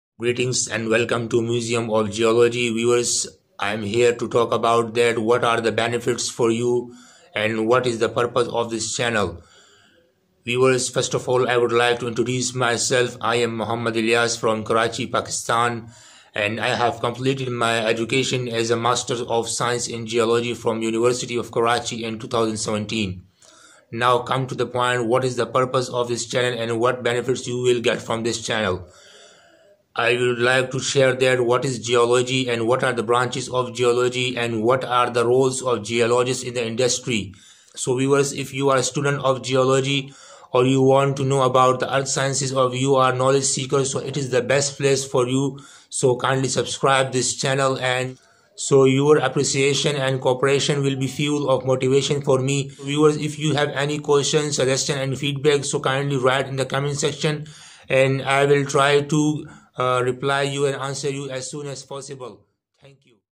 Best and amazing benefits from Museum of Geology for you || Why you should subscribe My Channel?
Greetings Viewers ||

This is the introduction about Museum of geology, what is the purpose of this channel? What are the benefits for you from Museum of geology? What will you get from Museum of geology?

You will get amazing and premium knowledge about geology, branches of geology, Roles of geologist in industry, Applications of geology and scope of geology in the industry. If you are student of geology, or knowlesdge seeker about geology and earth sciences so it is the best place and channel for you to subscribe it and enjoy the knowledge about earth sciences free of cost.

Do you want to grow your business by result-oriented digital and social media marketing?